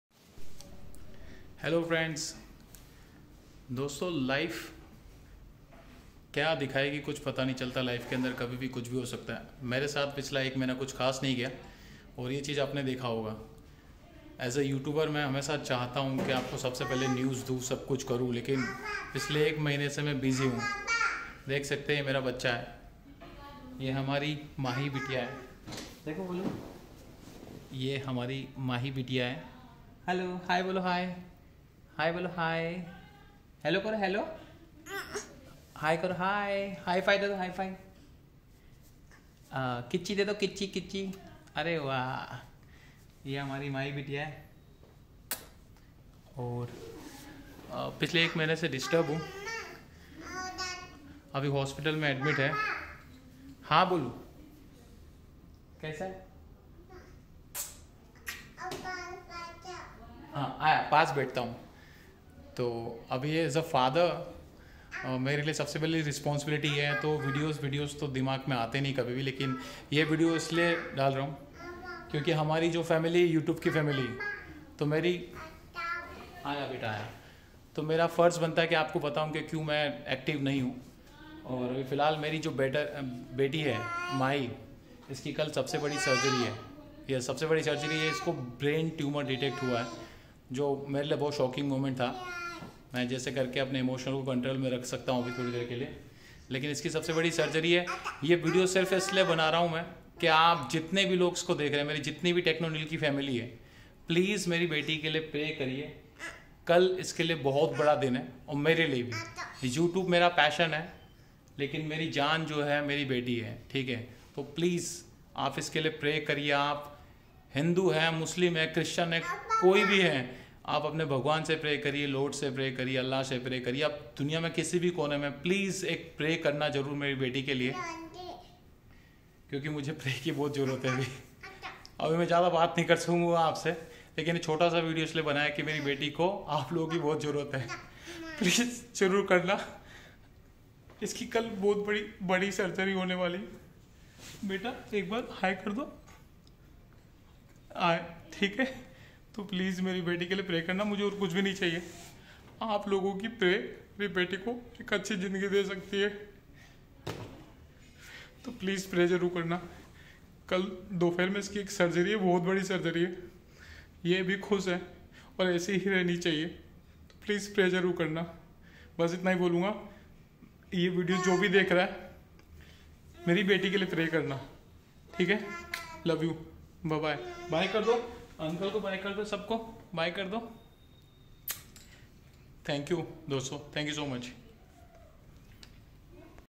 Tomorrow is My Daughter Brain 🧠 Tumor Big Surgery | Very Critical Time For Me 😭 Please Pray For Mahi
Hey Friends Tomorrow is My Daughter Brain 🧠 Tumor Big Surgery at CIMs Hospital Ahmedabad At 2 PM So it's Very Critical Time For My Cute Little Princess And Very Tuff Time For Me And My Family So Please Give Your Love And Support To My Daughter And Please Pray For My Daughter That She Will Be Fine Soon And The Surgery Will Go Sucessful, Please Pray To Your God For Mahi 😭😭😭